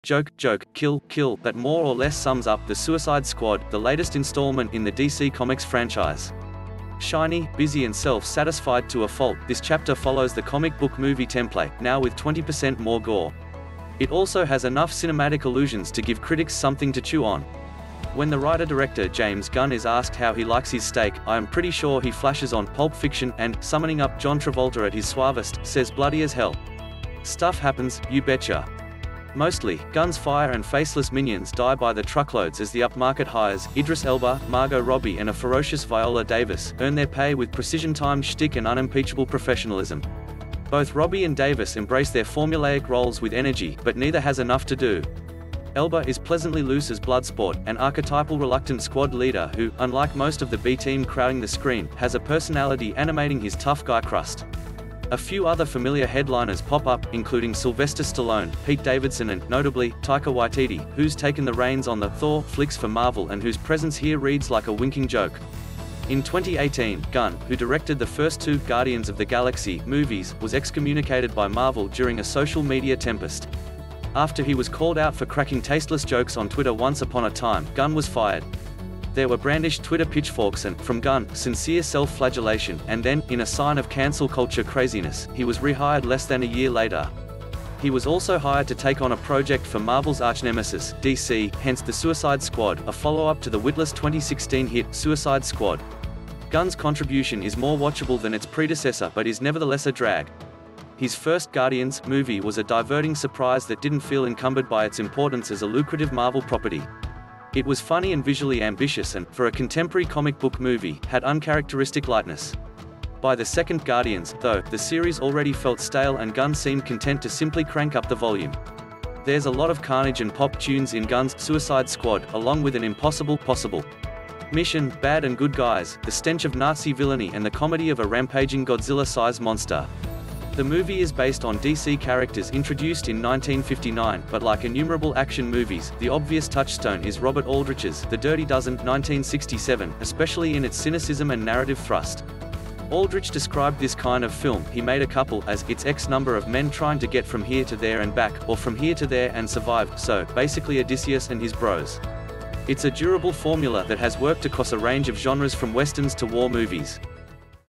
‘The Suicide Squad’ Review Train Them! Excite Them! Arm Them!
Joke, joke, kill, kill — that more or less sums up “The Suicide Squad, ” the latest installment in the DC Comics franchise. Shiny, busy and self-satisfied to a fault, this chapter follows the comic-book movie template, now with 20 percent more gore. It also has enough cinematic allusions to give critics something to chew on. When the writer-director James Gunn is asked how he likes his steak, I am pretty sure he flashes on “Pulp Fiction” and, summoning up John Travolta at his suavest, says “Bloody as hell. ”Stuff happens, you betcha. Mostly, guns fire and faceless minions die by the truckloads as the upmarket hires — Idris Elba, Margot Robbie and a ferocious Viola Davis — earn their pay with precision-timed shtick and unimpeachable professionalism. Both Robbie and Davis embrace their formulaic roles with energy, but neither has enough to do. Elba is pleasantly loose as Bloodsport, an archetypal reluctant squad leader who, unlike most of the B-team crowding the screen, has a personality animating his tough-guy crust. A few other familiar headliners pop up, including Sylvester Stallone, Pete Davidson and, notably, Taika Waititi, who’s taken the reins on the “Thor” flicks for Marvel and whose presence here reads like a winking joke. In 2018, Gunn — who directed the first two “Guardians of the Galaxy” movies — was excommunicated by Marvel during a social-media tempest. After he was called out for cracking tasteless jokes on Twitter once upon a time, Gunn was fired. There were brandished Twitter pitchforks and, from Gunn, sincere self-flagellation; and then, in a sign of cancel culture craziness, he was rehired less than a year later. He was also hired to take on a project for Marvel’s archnemesis, DC, hence “The Suicide Squad, ” a follow-up to the witless 2016 hit “Suicide Squad. ” Gunn’s contribution is more watchable than its predecessor but is nevertheless a drag. His first “Guardians” movie was a diverting surprise that didn’t feel encumbered by its importance as a lucrative Marvel property. It was funny and visually ambitious and, for a contemporary comic-book movie, had uncharacteristic lightness. By the second “Guardians, ” though, the series already felt stale and Gunn seemed content to simply crank up the volume. There’s a lot of carnage and pop tunes in Gunn’s “Suicide Squad, ” along with an impossible (possible! ) mission, bad and good guys, the stench of Nazi villainy and the comedy of a rampaging Godzilla-size monster. The movie is based on DC characters introduced in 1959, but like innumerable action movies, the obvious touchstone is Robert Aldrich’s “The Dirty Dozen” (1967), especially in its cynicism and narrative thrust. Aldrich described this kind of film (he made a couple) as “it’s X number of men trying to get from here to there and back, or from here to there and survive” — so, basically Odysseus and his bros. It’s a durable formula that has worked across a range of genres from westerns to war movies.